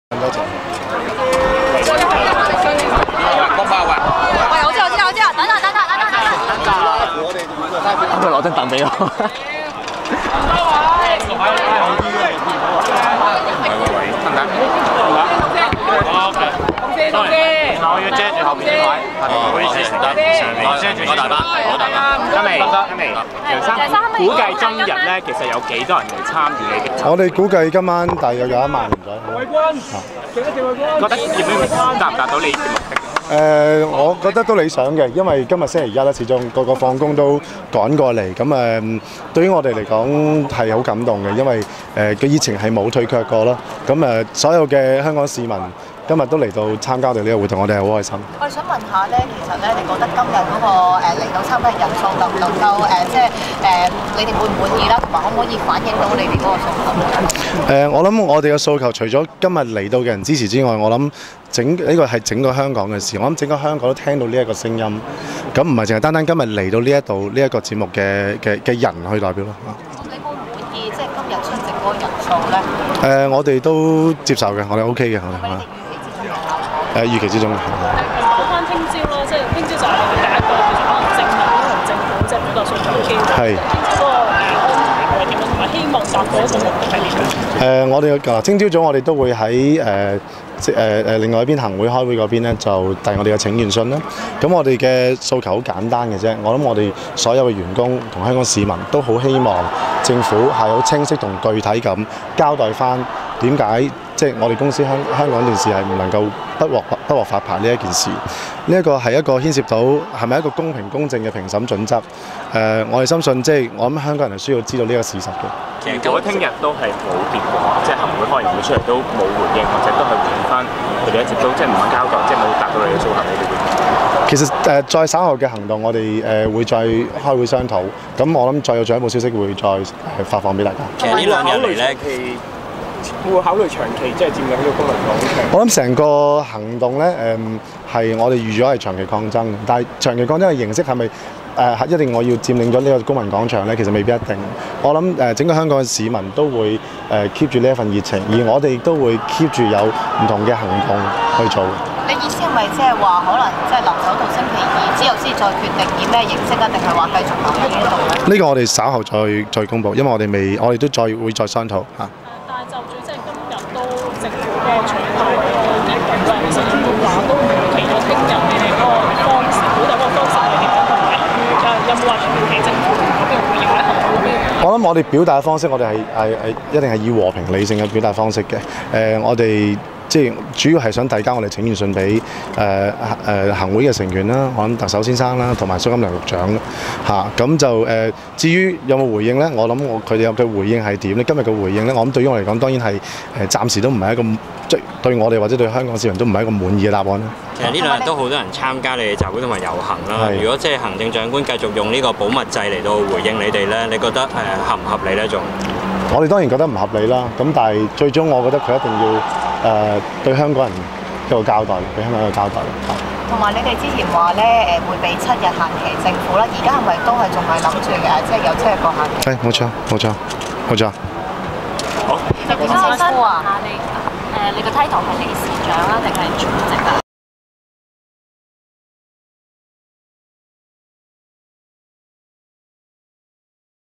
[10月21日]新聞第一線 - 港視職工會楊志豪：會留守到明早 暫未有意長佔政總
記者：文嘉豪
攝影：蔡雋熙

(__ Month) Category - Title
Reporter:
News Gathering/Videographer: (DELETE INAPPROPRIATE)